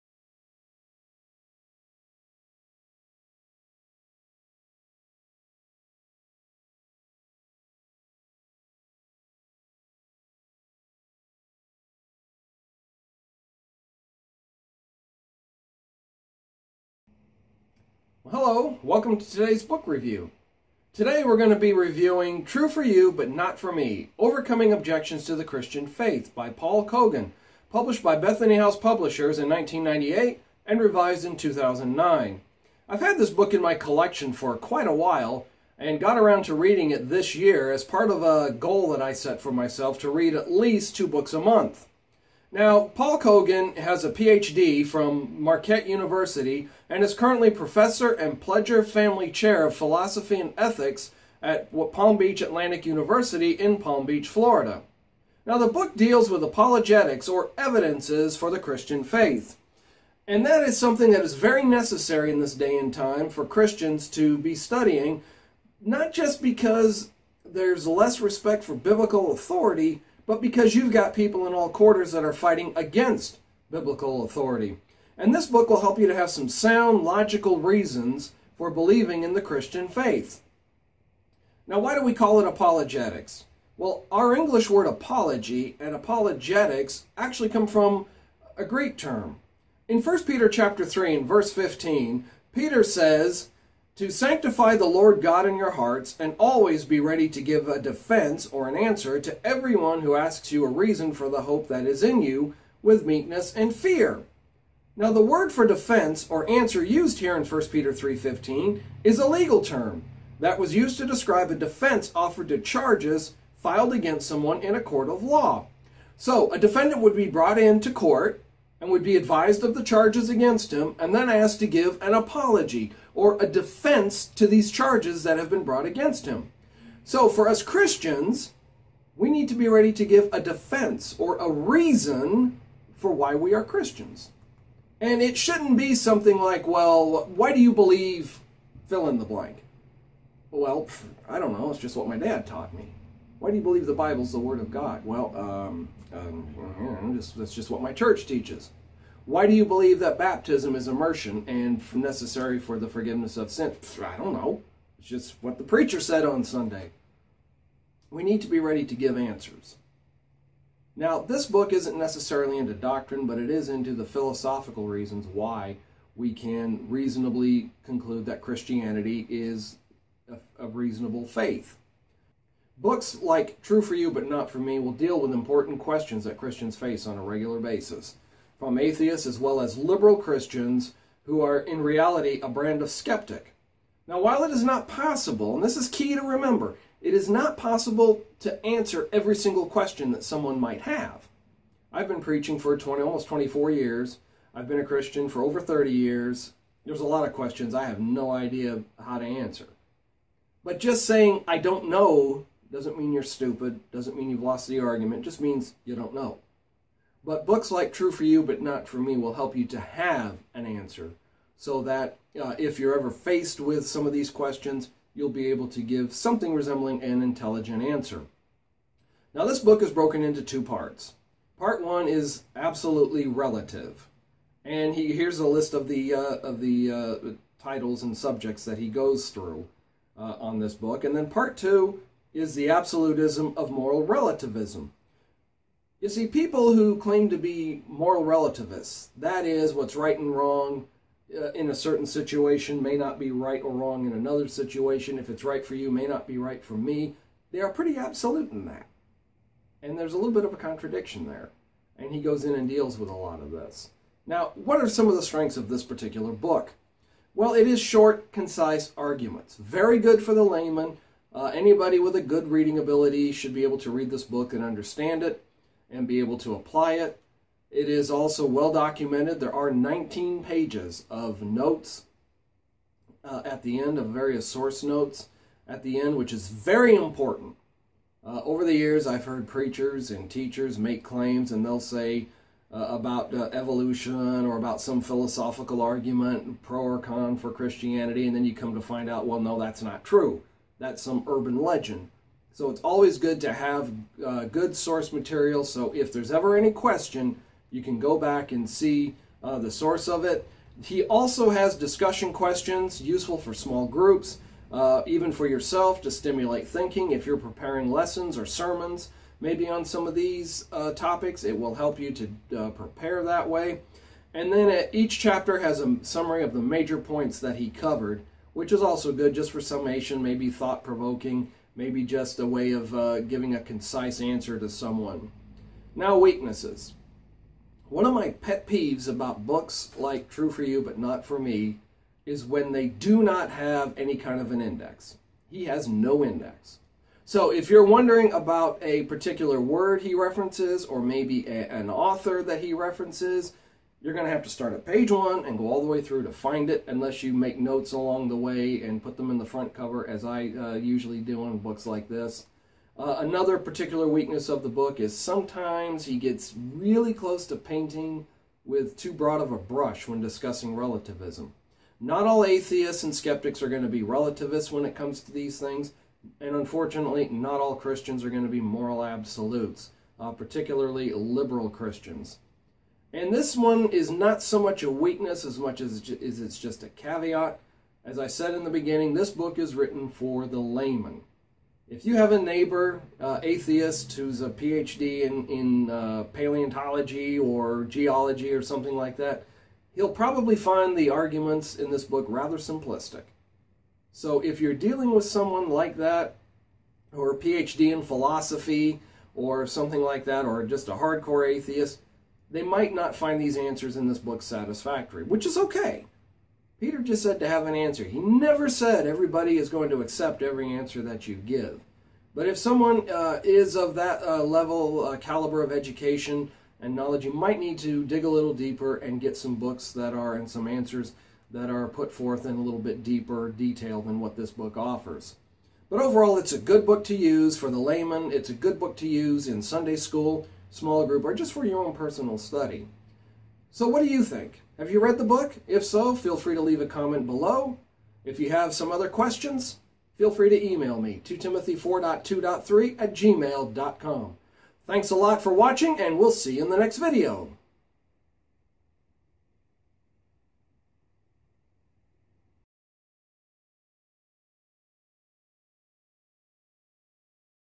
Book Review True For You But Not For Me: Overcoming Objections to the Christian Faith
My review of the 1998 edition of this book